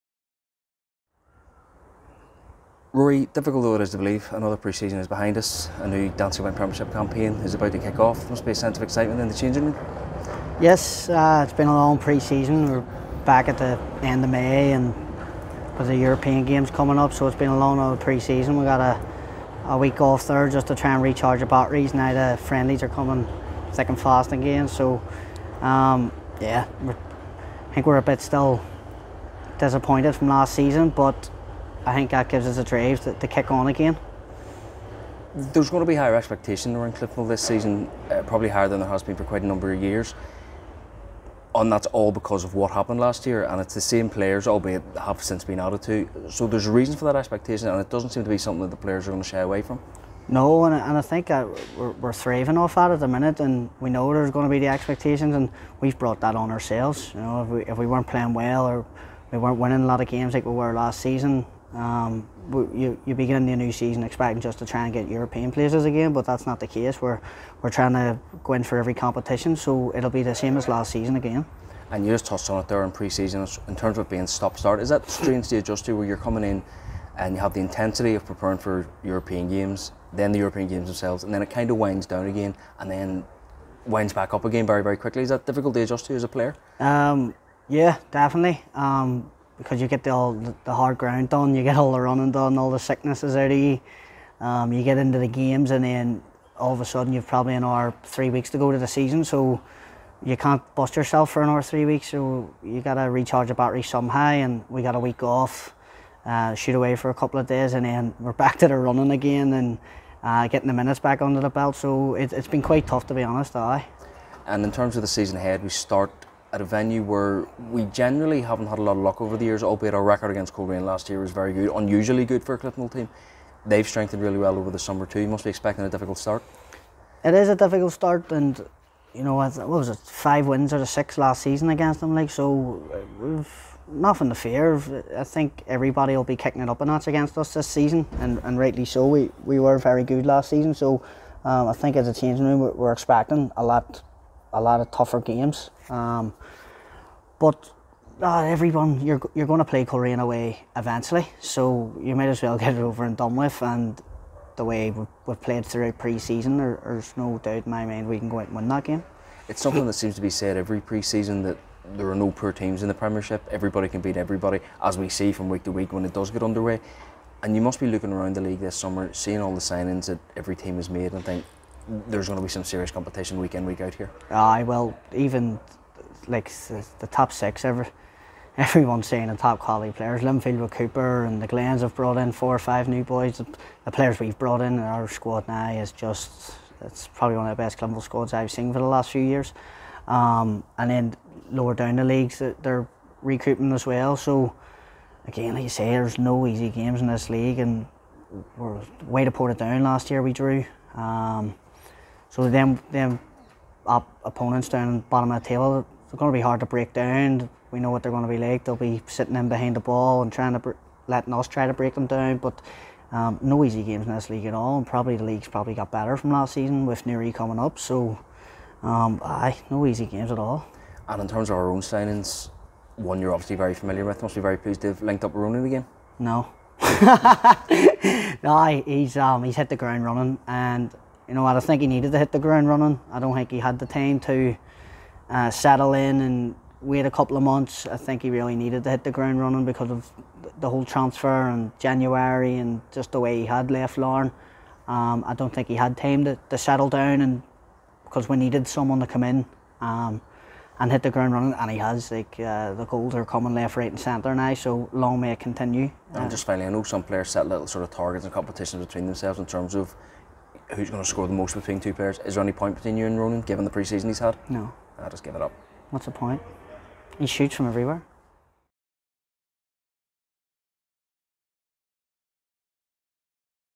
2022-23 COUNTDOWN | Rory Hale
Rory Hale expects Cliftonville to face stern challenges at every turn throughout the forthcoming Danske Bank Premiership campaign.